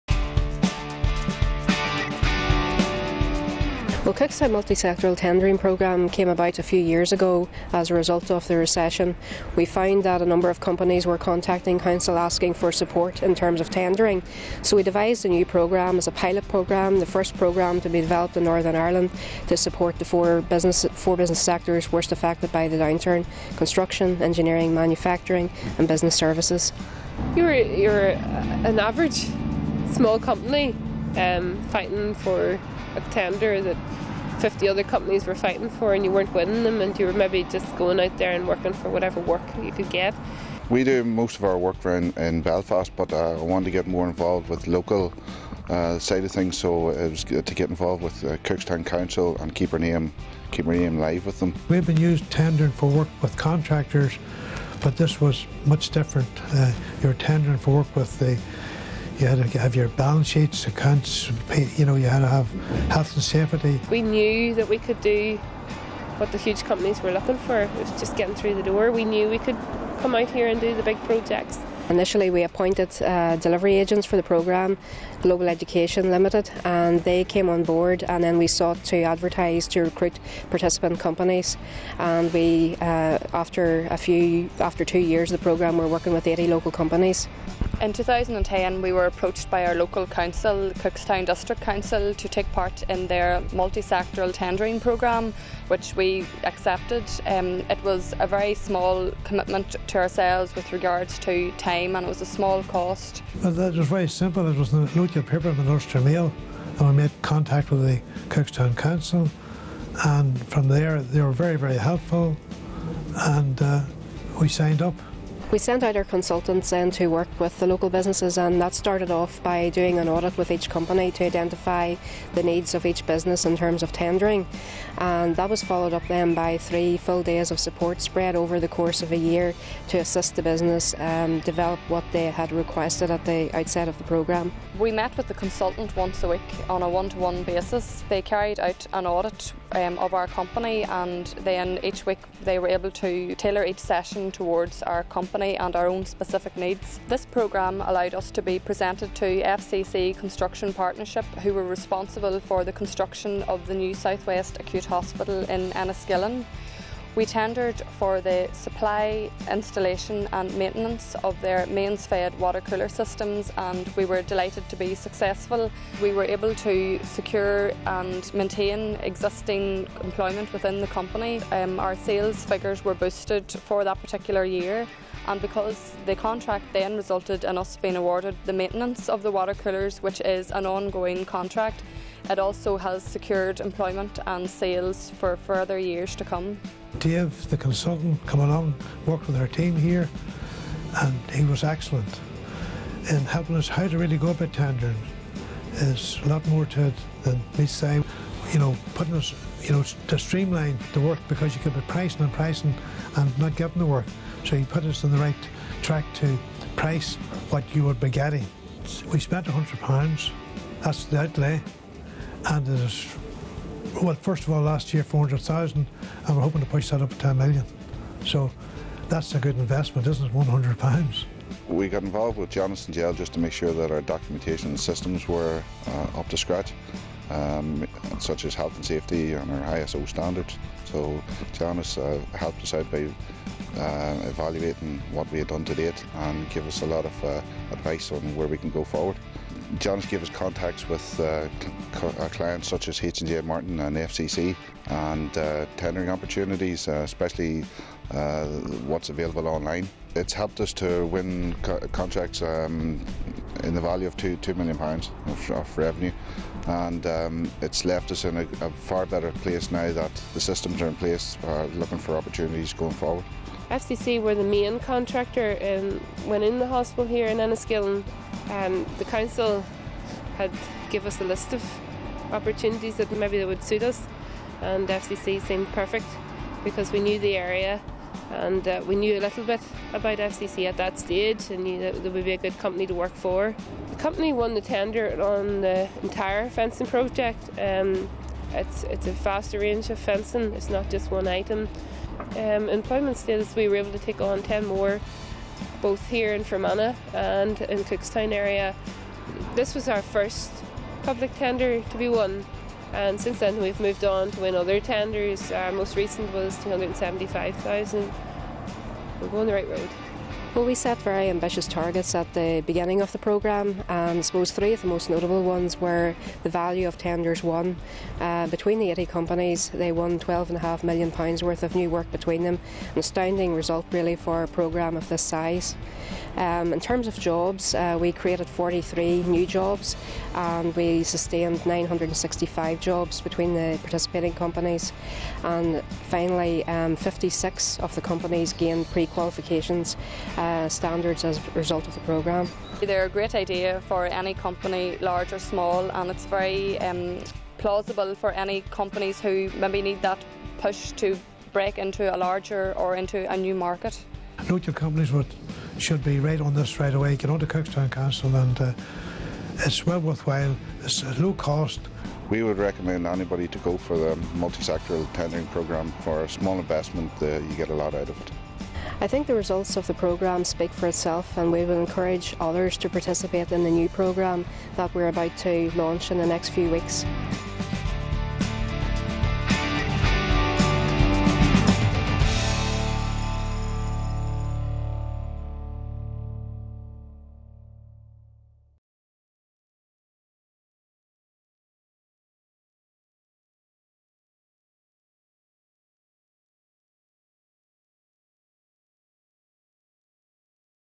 Cookstown District Councils Multi Sectoral Tendering Programme
Achievements of local Cookstown Businesses on Cookstown District Councils Multi- Sectoral Tendering Programme.  Hear from Rocwell Natural Mineral Water, IL Contracts, CMS Pipelines & Forbes Contracts.